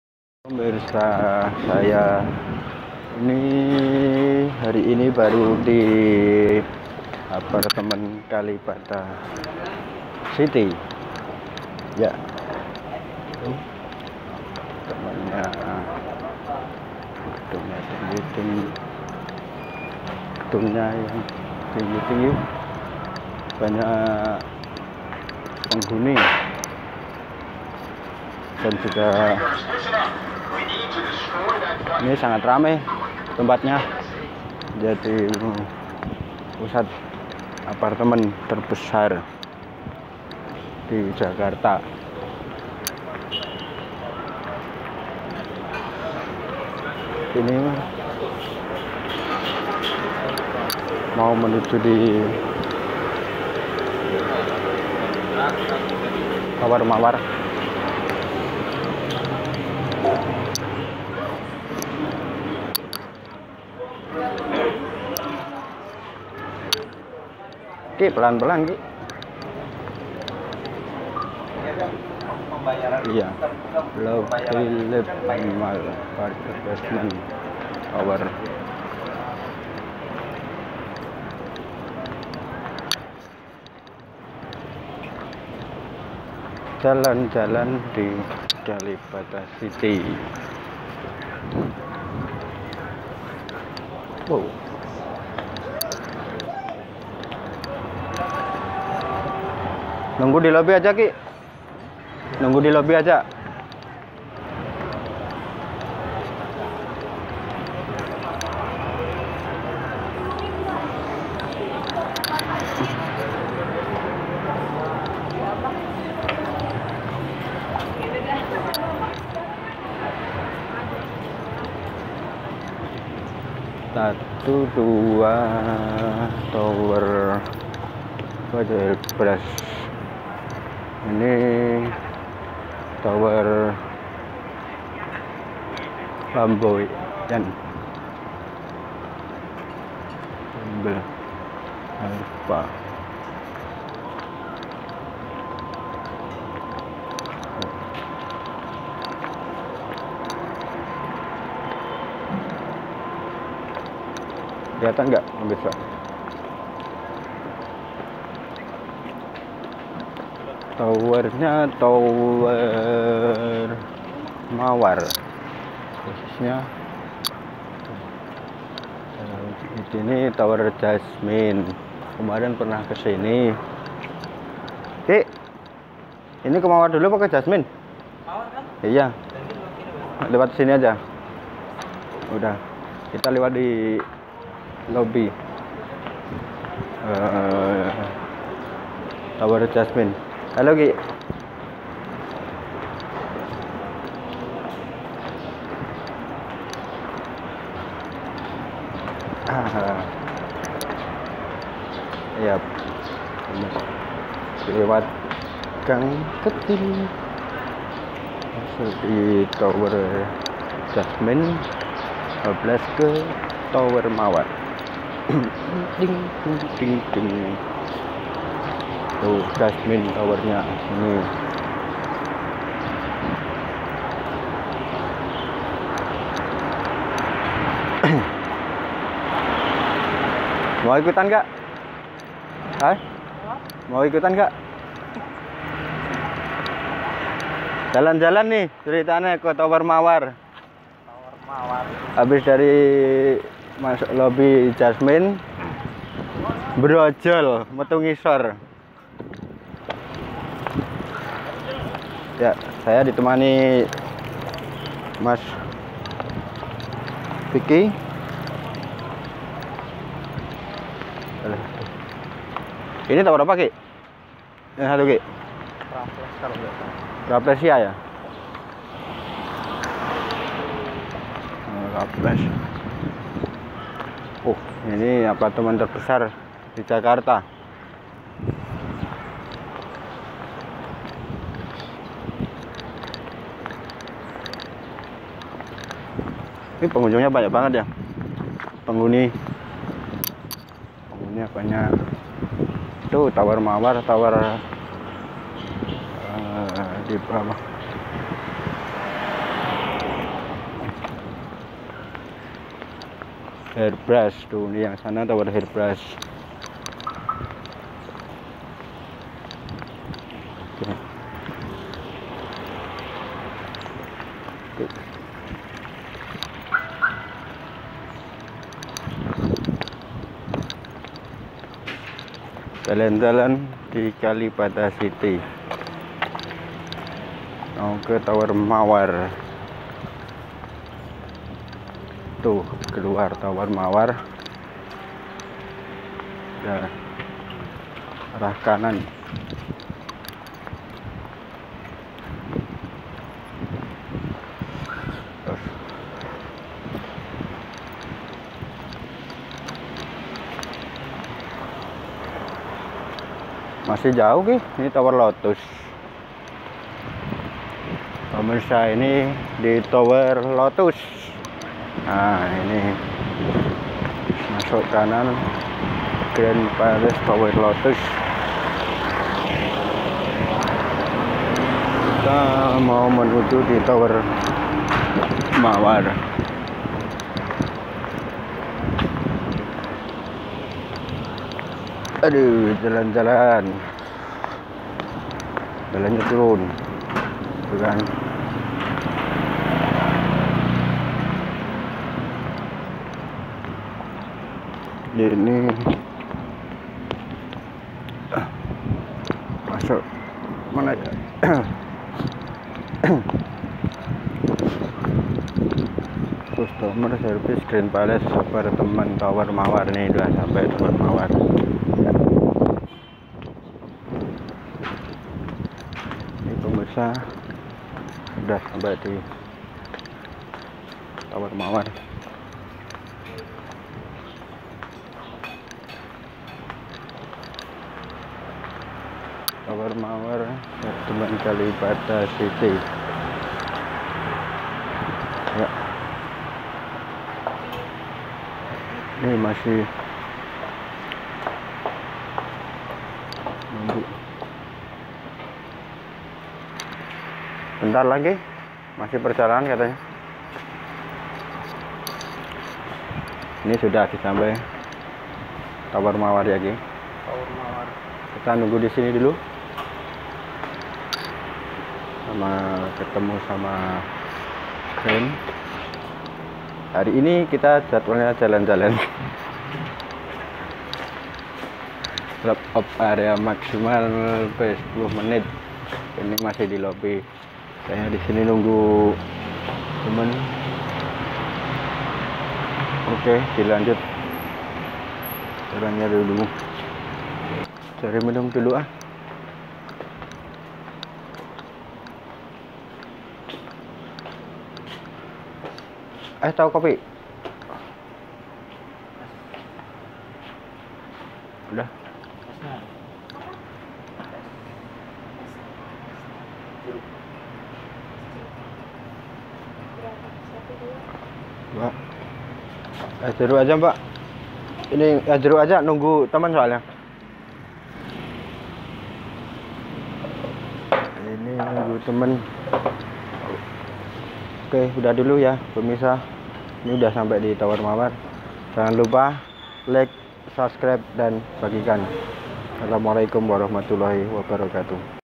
Vlog Review Apartemen kalibata City Tower Mawar
jalan-jalan di apartemen kalibatacity menuju tower mawar.
Review lokasi apartemen kalibata city